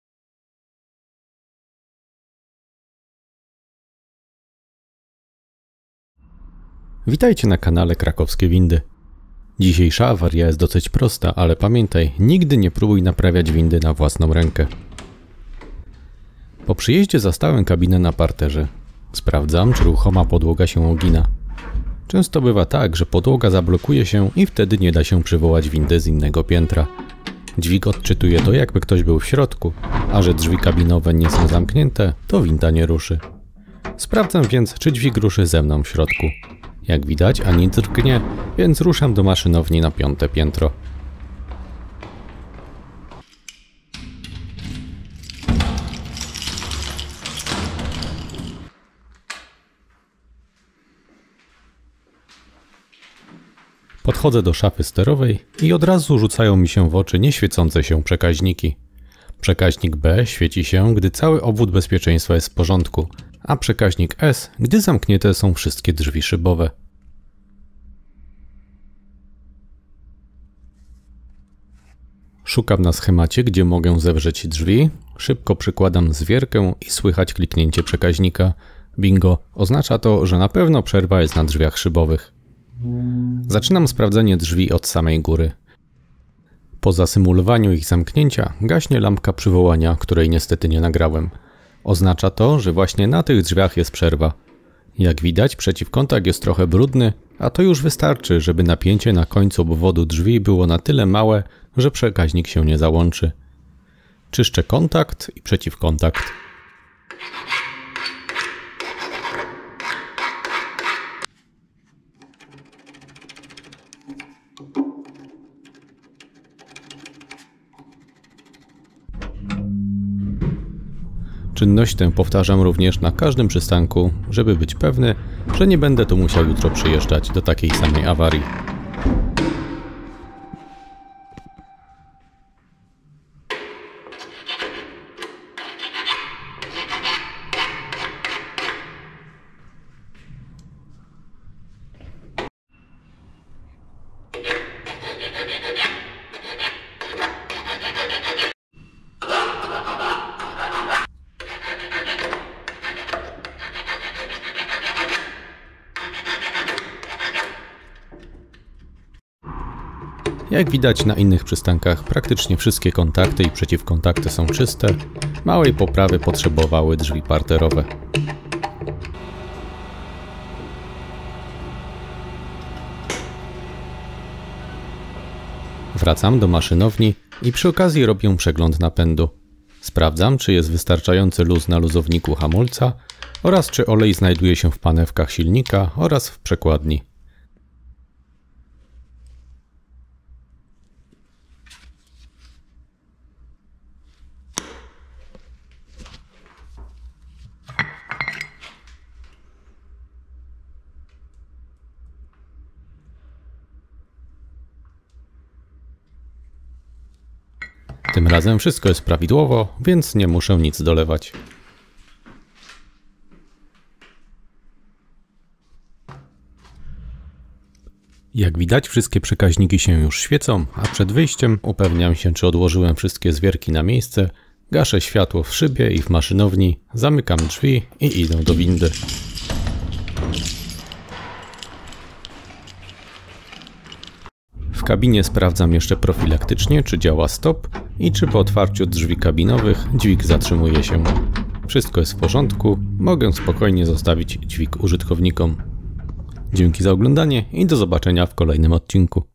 Awaria windy - drzwi zamknięte, a jakby otwarte.
Klasyczna i dosyć częsta awaria. Tym razem poszło szybko, ale czasami trzeba się naszukać, zwłaszcza w wieżowcach.